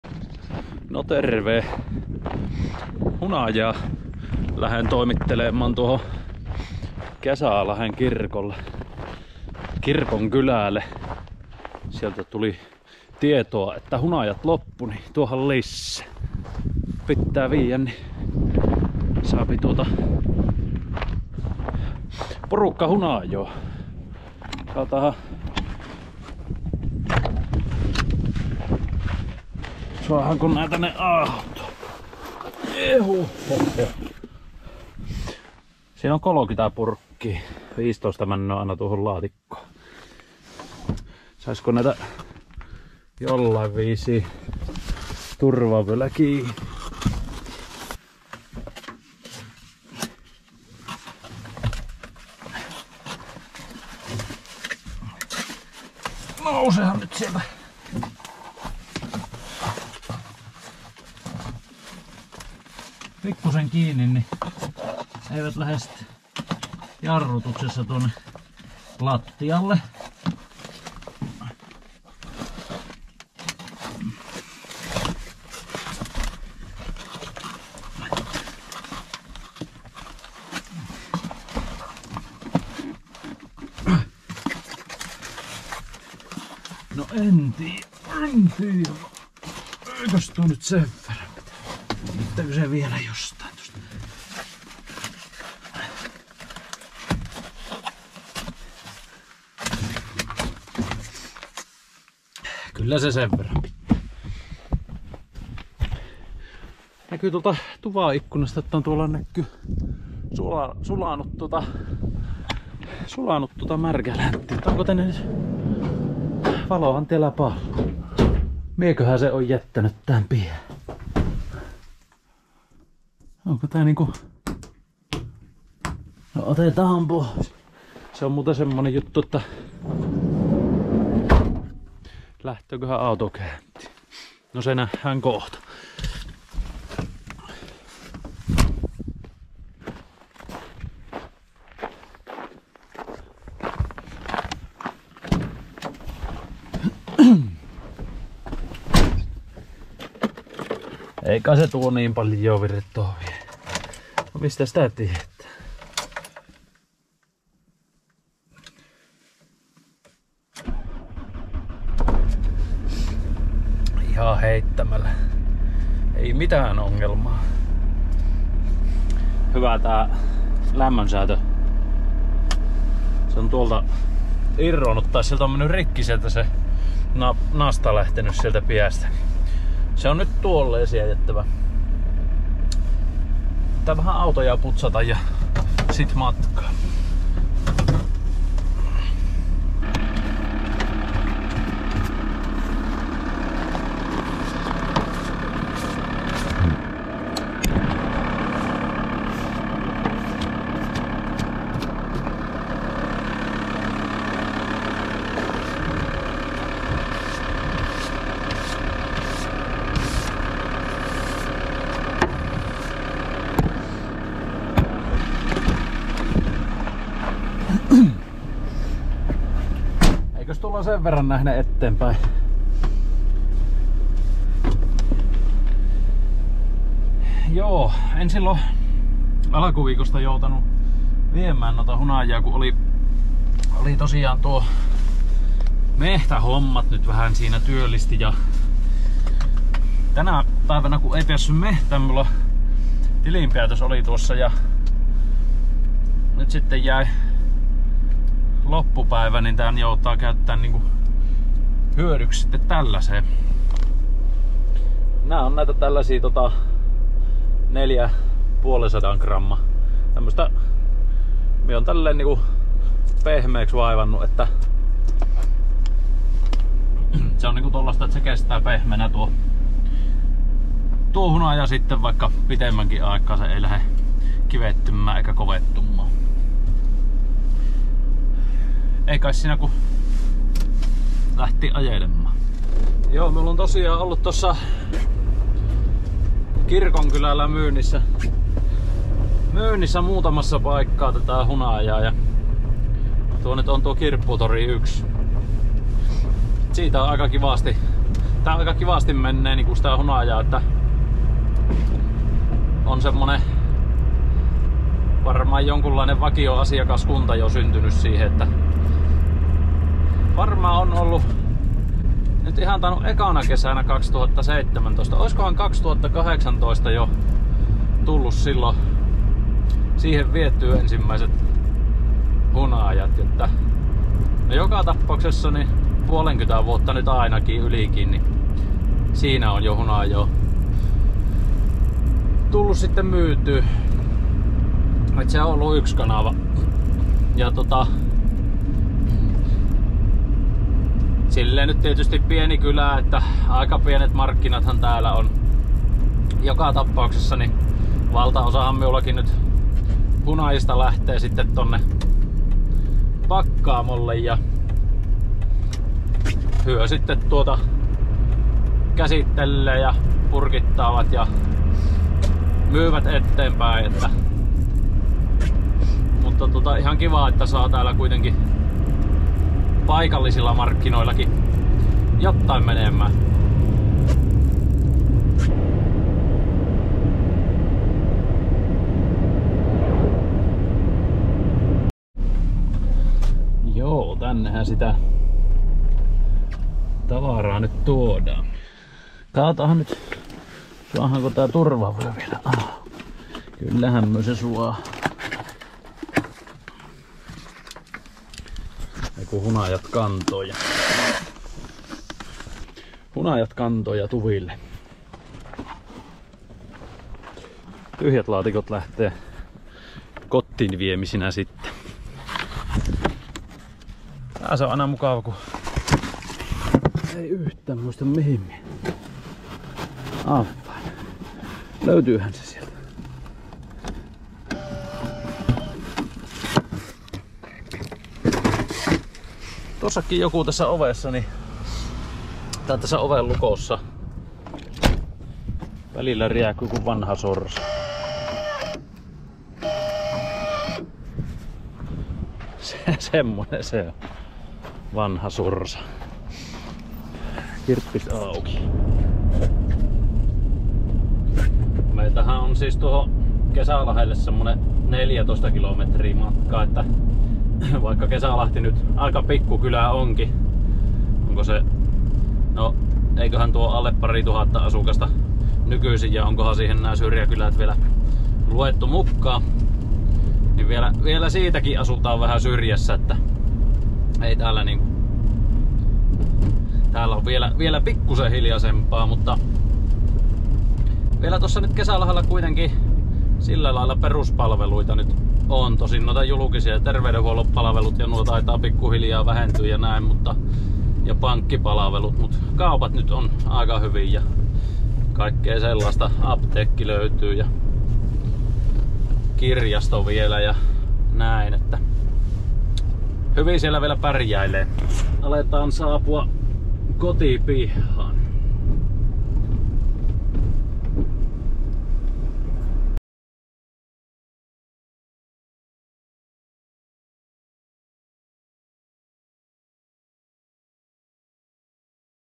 Keskiviikon iltapäivänä hunajat kirkonkylälle
Lyhyt reissu Tranporterilla hunajavarastojen täydennykselle Kesälahden keskustan kirpparin myyntipisteelle. Höpöttelyä hunajasta ja hunajan vierestä.

Vlogi sääriamputoidun maanviljelijän elämästä Pohjois-Karjalassa, työstä, harrastuksista ja haasteista.